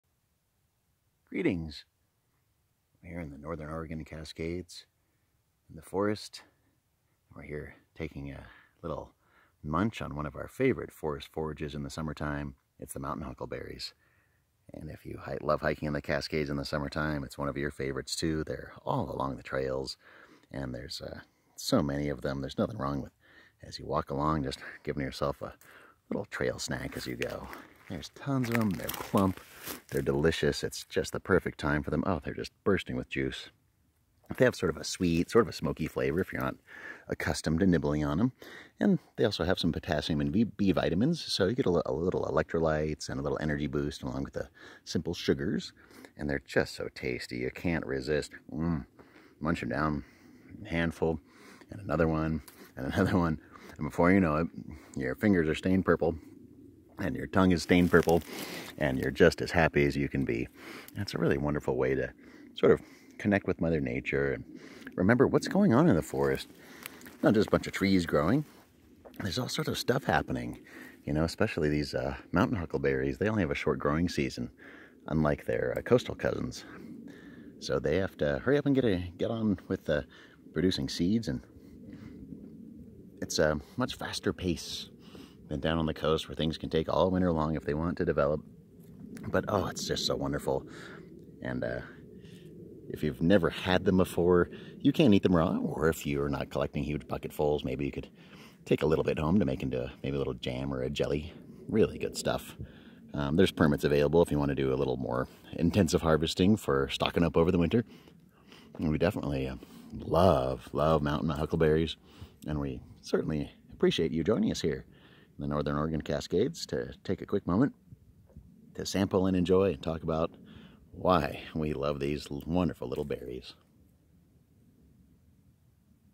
Good Plants: mountain huckleberry - Always Ants
Join us in the northern Oregon Cascades as we take a tasty look at a favorite summertime treat, mountain huckleberry, in August 2019. This abundant and delicious berry is a welcome sight every summer in these mountains, and anyone who spends time here can't help but end up with at least a few fingers stained purple. Along with a simple sugar hit, huckleberries are nutritious, with B vitamins and potassium, making them the perfect snack as you hike through these beautiful mountain forests!